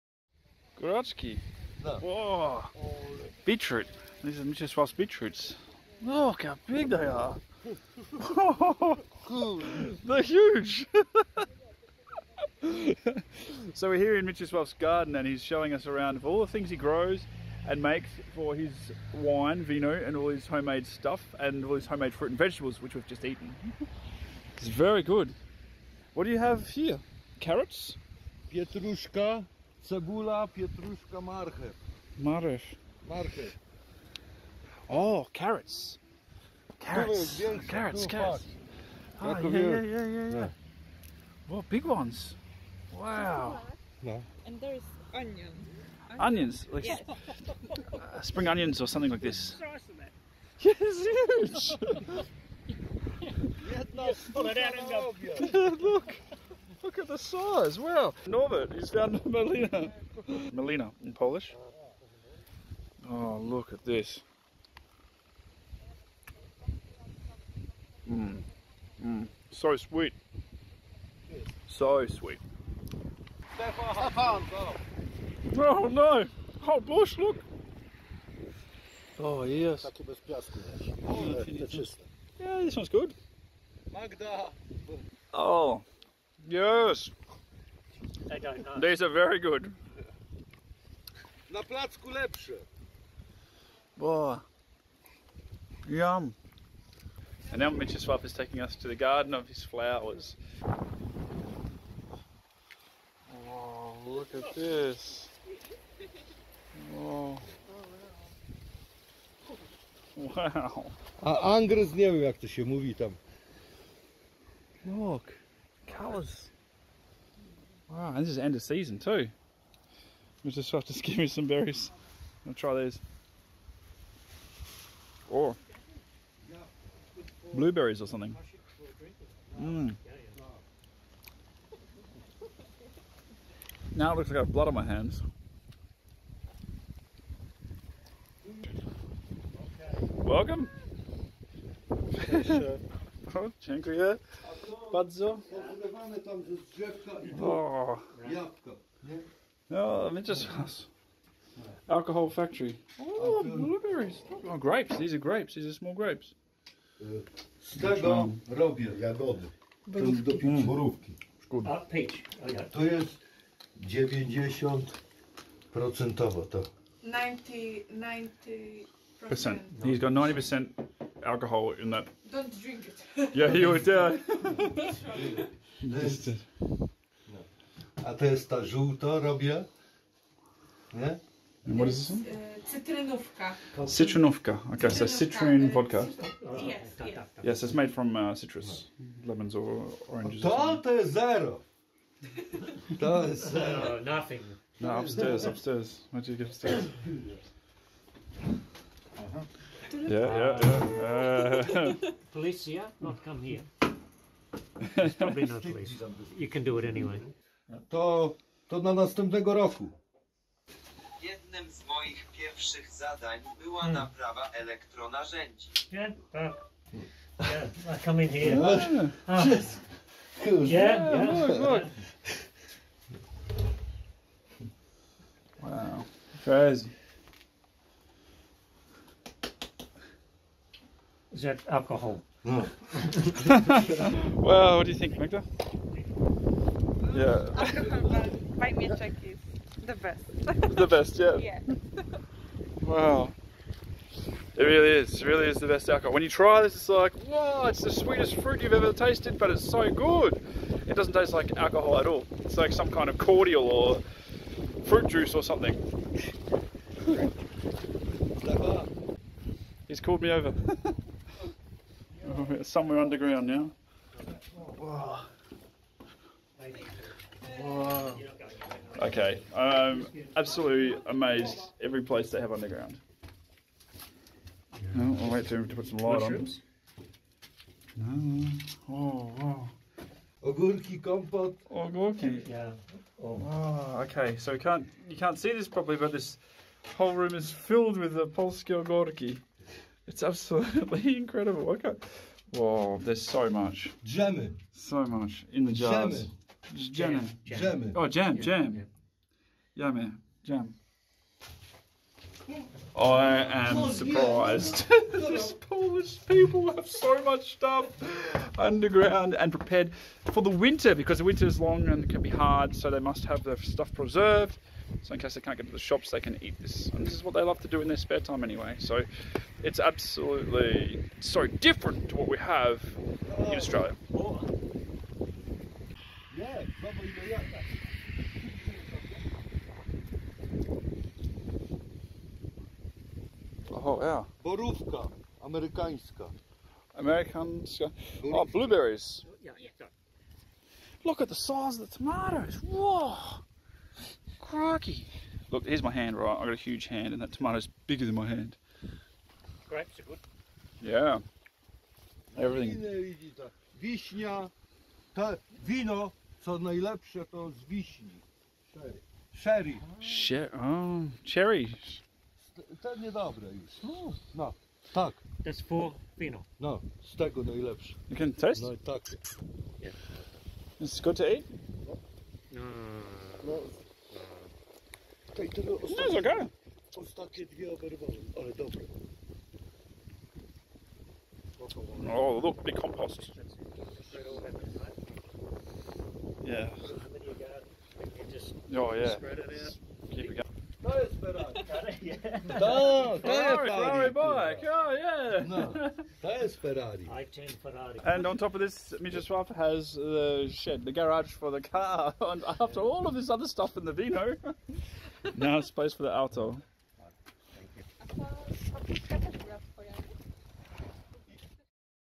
Remembering Mieczyslaw - Garden Delights, Homemade Wine and Steam Train Driver
This beautiful Polish garden belonged to Mieczyslaw, a passionate gardener who loved his plants, fruits, and veggies. He kindly showed us around his garden, where we could have stayed for a long time just enjoying the fresh produce. Mieczyslaw shared his secret recipe for making wine from his garden's produce and took us to his underground cellar filled with homemade food to get through the long, cold winters.

Sadly, Mieczyslaw has now passed away, but this video is a wonderful memory of his warm personality. Mieczyslaw was also involved in the Polish railways and was a driver of the steam locomotive  Piękna Helena, meaning "Beautiful Helena" and other locomotives at the Wolsztyn Museum. You can see more about this on my channel in the video about the last steam train service in the world.

Join us in celebrating Mieczyslaw's life and passions in this heartfelt video.